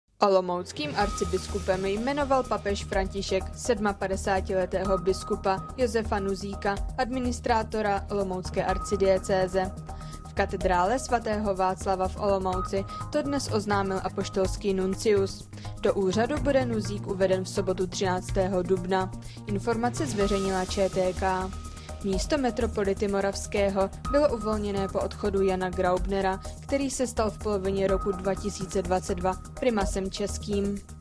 Josef Nuzík byl jmenován novým olomouckým arcibiskupem
Olomouckým arcibiskupem jmenoval papež František sedmapadesátiletého biskupa Josefa Nuzíka, administrátora olomoucké arcidiecéze. V katedrále svatého Václava v Olomouci to v pátek oznámil apoštolský nuncius v ČR. Do úřadu bude Nuzík uveden v sobotu 13. dubna. Informaci zveřejnila ČTK. Místo metropolity moravského bylo uvolněné po odchodu Jana Graubnera, který se stal v polovině roku 2022 primasem českým.